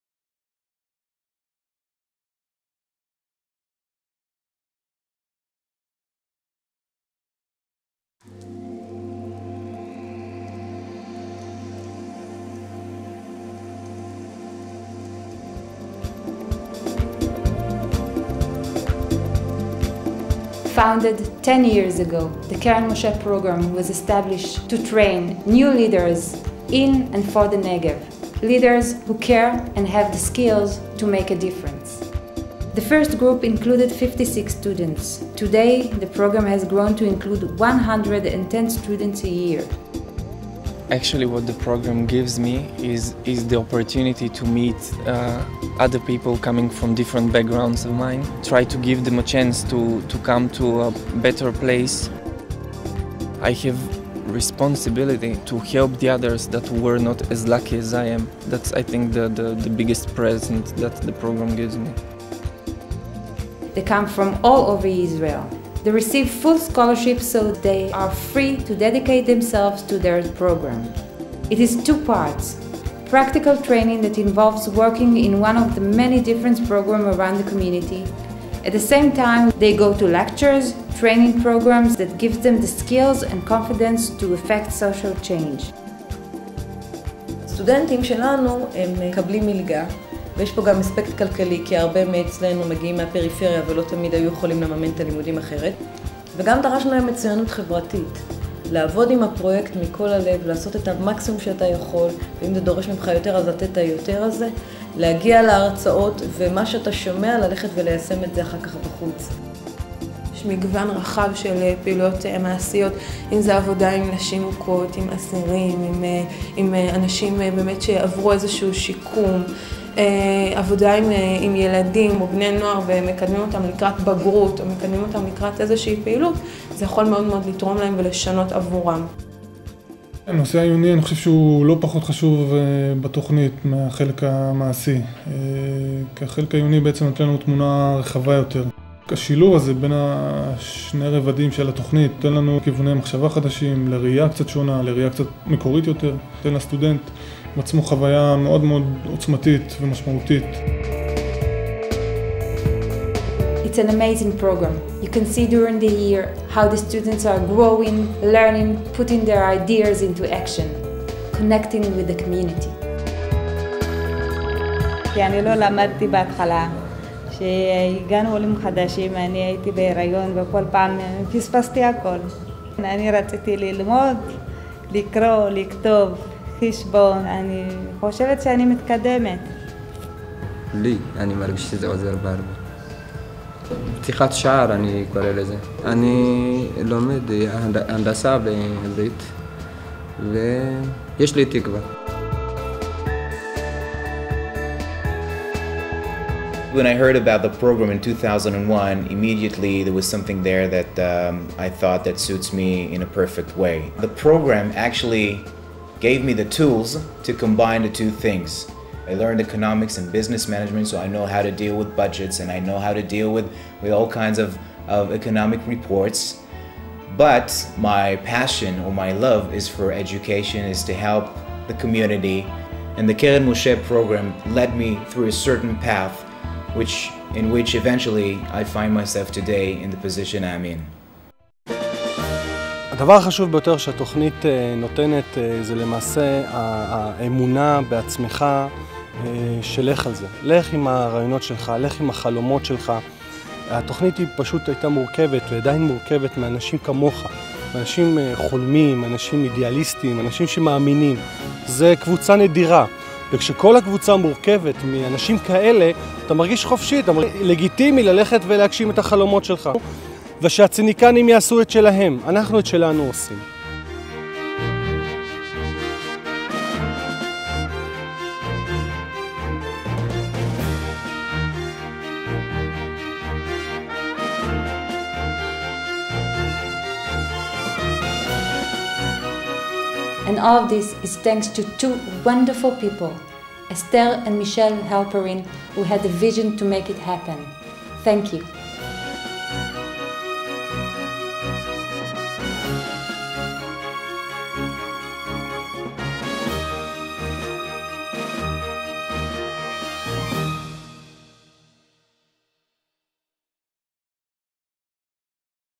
The Keren Moshe Leadership Training Program - English version
Produced in 2008, this movie is about the 10th Anniversary of the Community Action Unit's Keren Moshe Leadership Training Program.